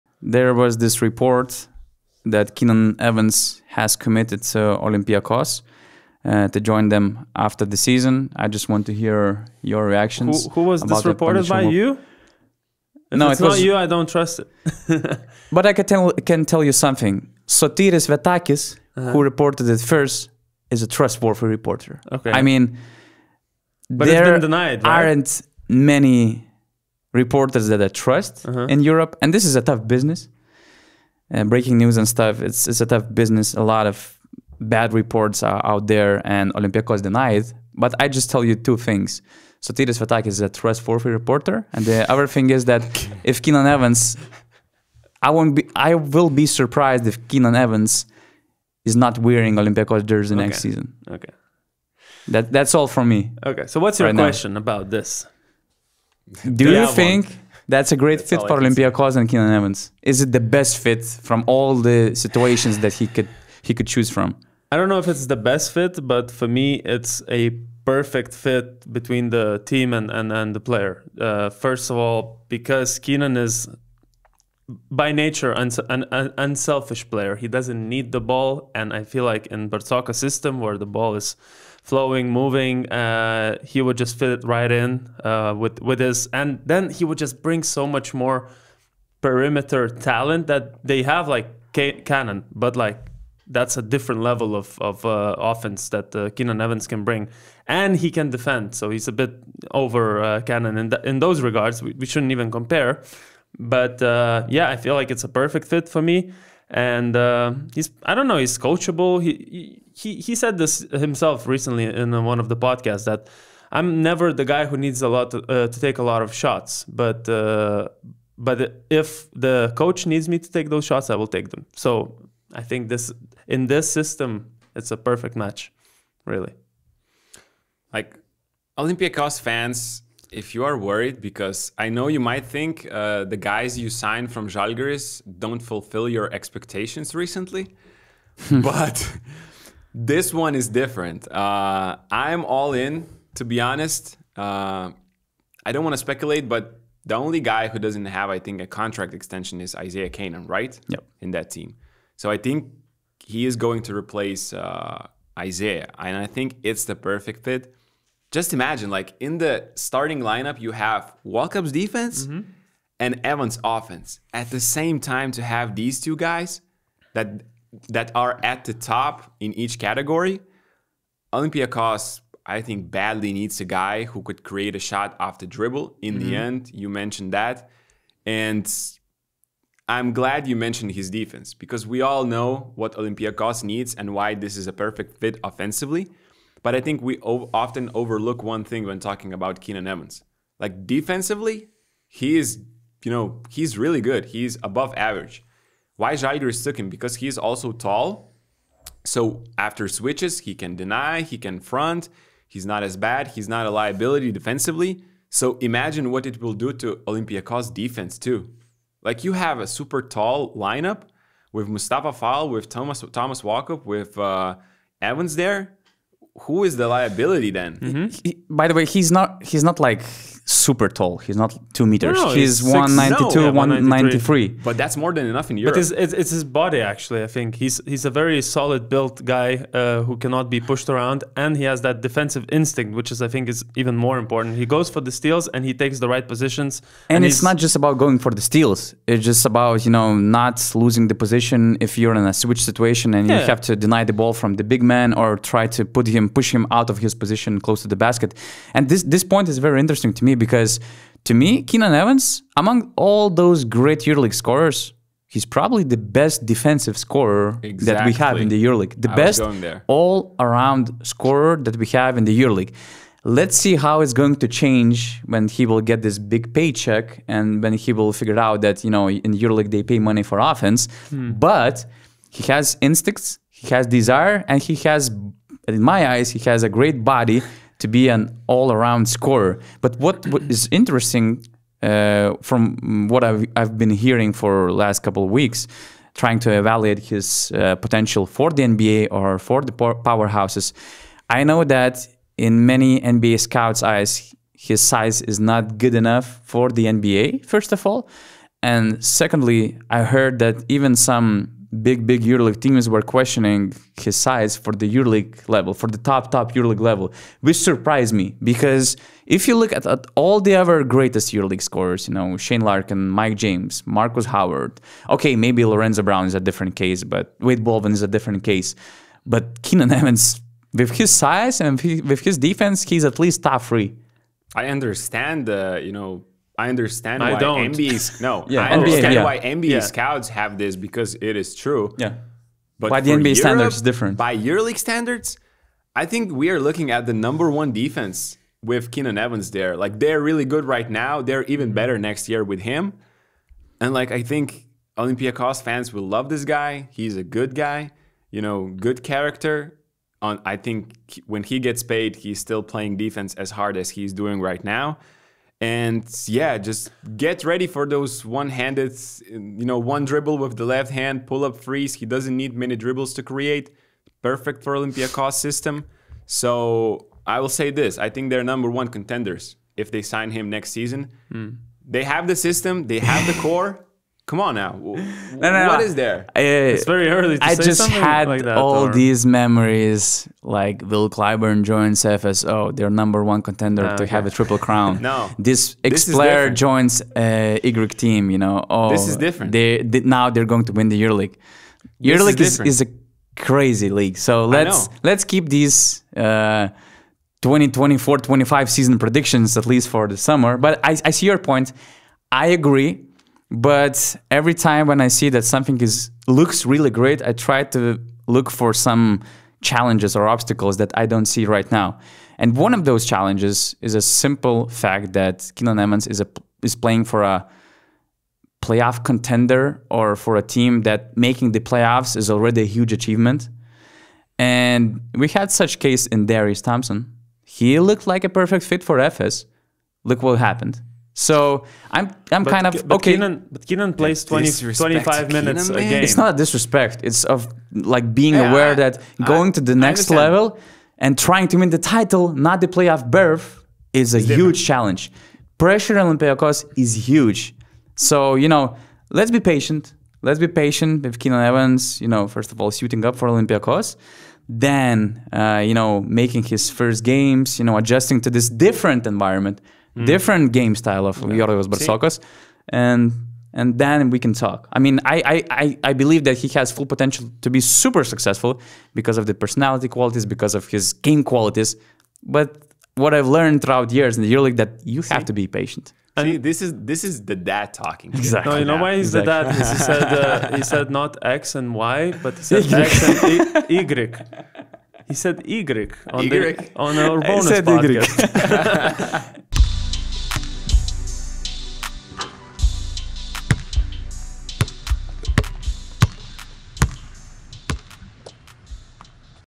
Would Evans Make Olympiacos The #1 EuroLeague Contender?
URBONUS’ Donatas Urbonas, Gytis Blazevicius & Augustas Suliauskas explore the rumors of Keenan Evans joining Olympiacos and would that make Olympiacos a No 1 contender to win the EuroLeague title?